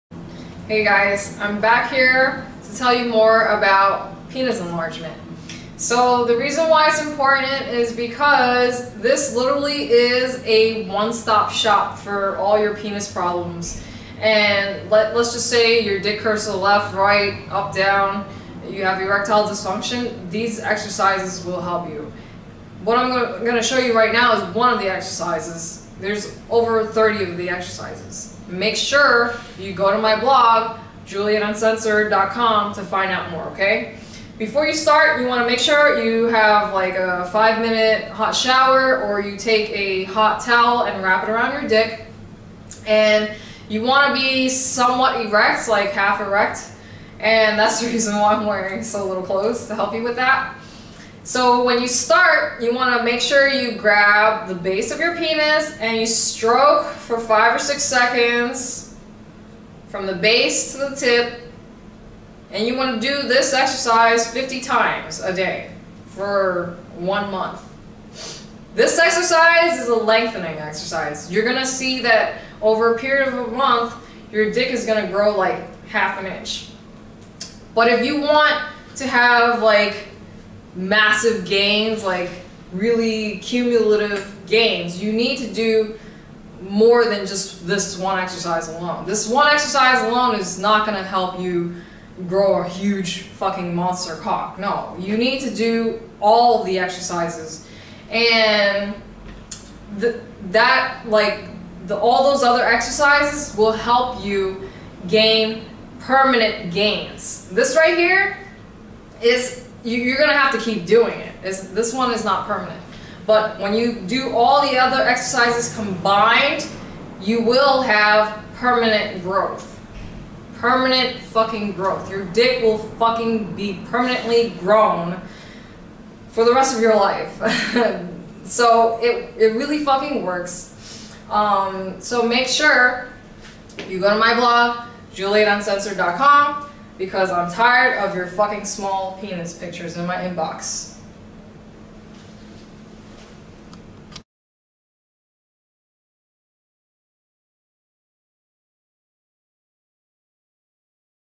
Tiny Dick Problems- Grow Your Penis with this Penis Enlargement Trick
How To enlarge your penis.  Grow your Penis you piece of shit.  Wake the fuk you up micropenis'd dudes.  This shit fucking works.  You will increase the size of your penis.  It's not jacking off your penis.  This is an exercise to help you get a bigger cock.  I'm tired of getting these baby dick pictures from random guys.  Don't you want to have a big dick so that people will stop making fun of you?  I'm trying to help you, but you can't be helped unless you wake the fuck up and wanna HELP YOURSELF by growing your pathetic limp dick.  I'm tired of dealing with niggas with small broke dicks and can't even get an erection.  The worst is when a tiny dicklet is so small that he slips out and the condom gets lost.  Like what the fuck dude I ain't got time for you little dicks.  Seriously.  I'm helping you because I'm indirectly helping females.  Go to my blog and you will learn how to PERMANENTLY GROW YOUR PENIS.  Everything you need is right there.  You just have to read.
Growing your dick isn't about finding a partner.  Growing your penis is about self improvement.  When you feel better about yourself your confidence and self esteem will skyrocket.  Your perspective and your experience and outlook will do a complete 360.  You will be happier with not just your manhood, but your entire life.  Aren't you tired of bitches laughing about your small penis?  Don't you wish you had a huge cock that you could shove down a bitches throat to shut her the fuck up?  Don't you want to make her squirm with pleasure?  Or do you want to be a little bitch and let other people disrespect and mistreat you for the rest of your life?  You choose fellas.  I'm done here.